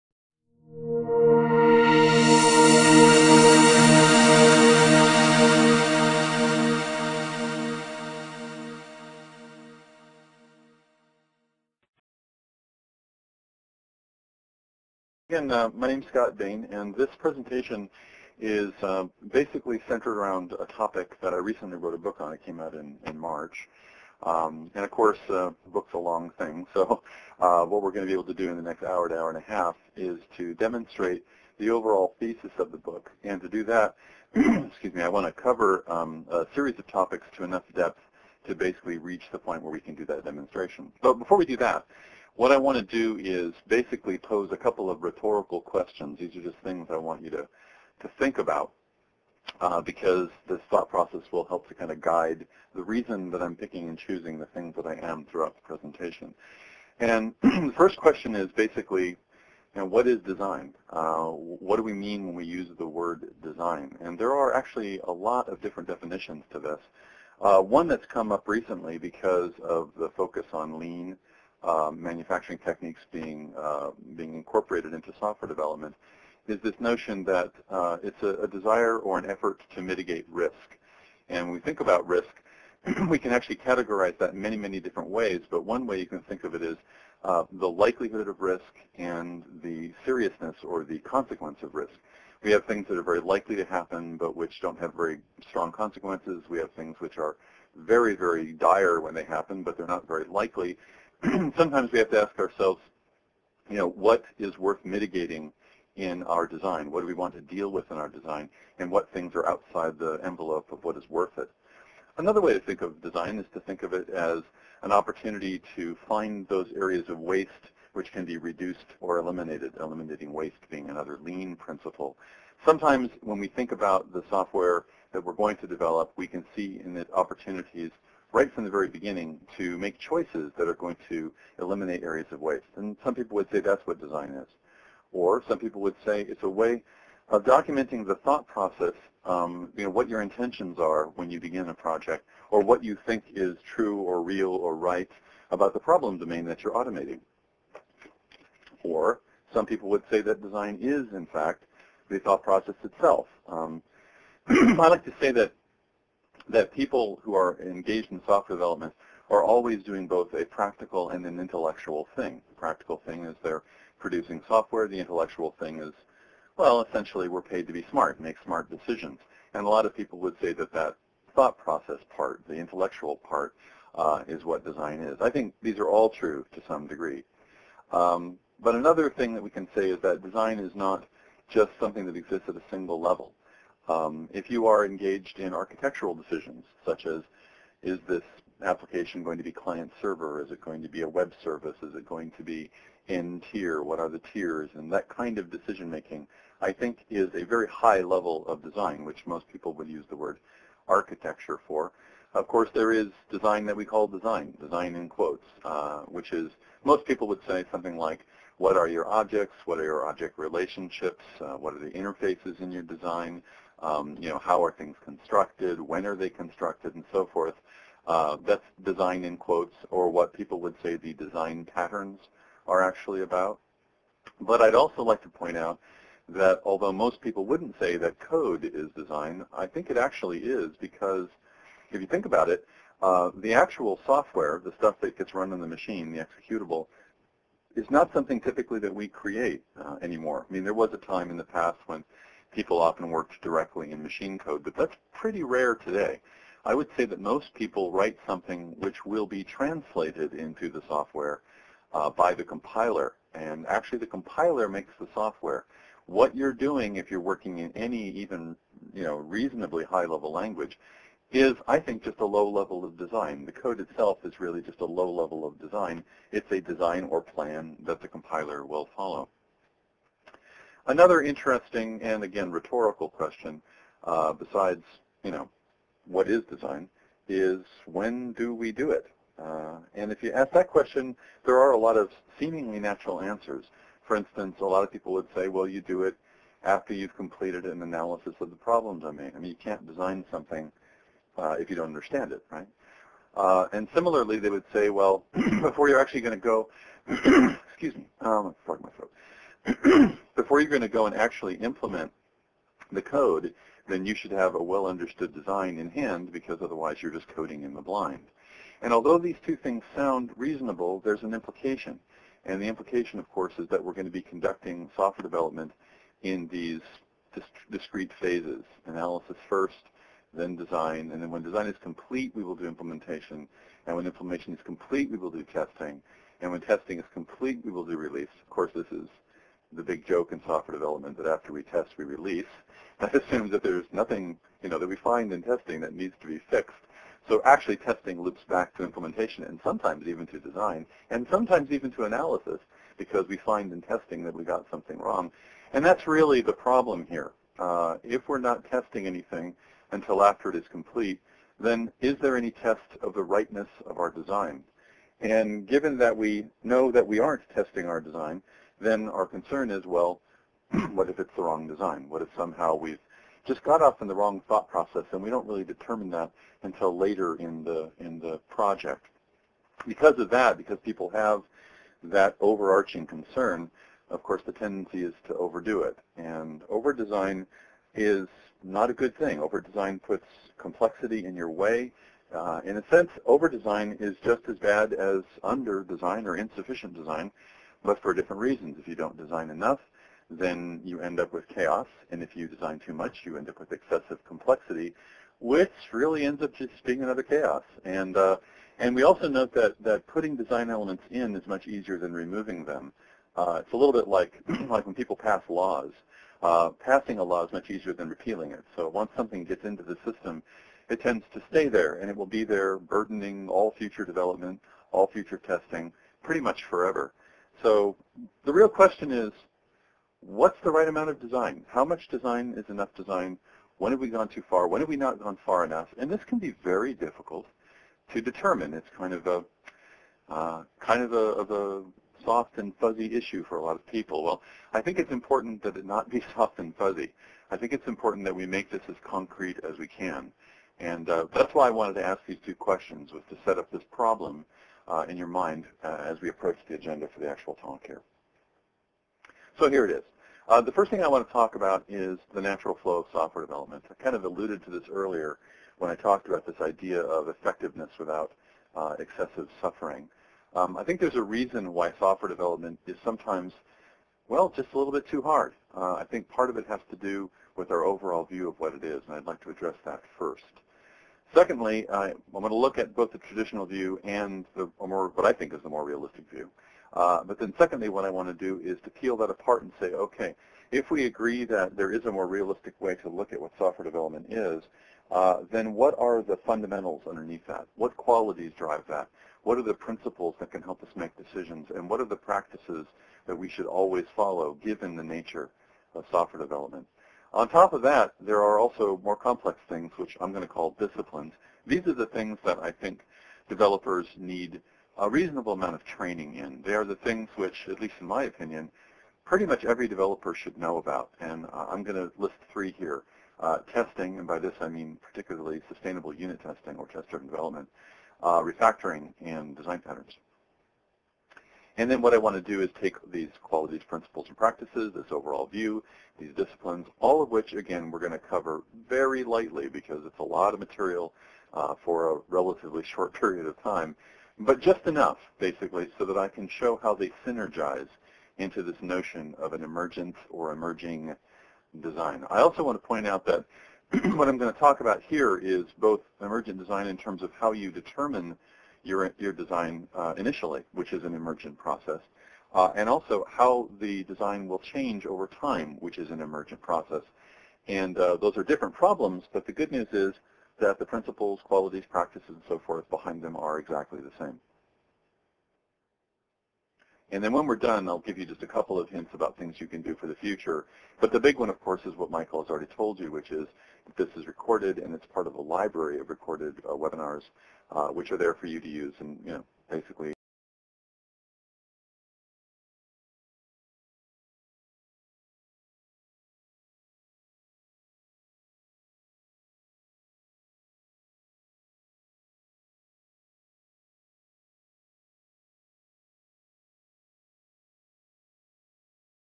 Emergent Design: The Professional Nature of Professional Software Development
What is design? When do we do it? Can we test design? How much design is too much? This webinar with Scott Bain addresses these questions by examining the essential nature of software development, and by offering a view of the essential principles, practices, and disciplines a team must know and apply to work efficiently in a changing world.

ABOUT THE PRESENTER:
Scott Bain is a 30+-year veteran in computer technology, with a background in development, engineering, and design. He has also designed, delivered, and managed training programs for certification and end-user skills, both in traditional classrooms and via distance learning. Scott teaches courses and consults on Agile Analysis and Design Patterns, Advanced Software Design, and Sustainable Test-Driven Development. Scott is a frequent speaker at developer conferences such as JavaOne and SDWest. He is the author of "Emergent Design: The Evolutionary Nature of Professional Software Development" now available from Addison Wesley/Pearson Education.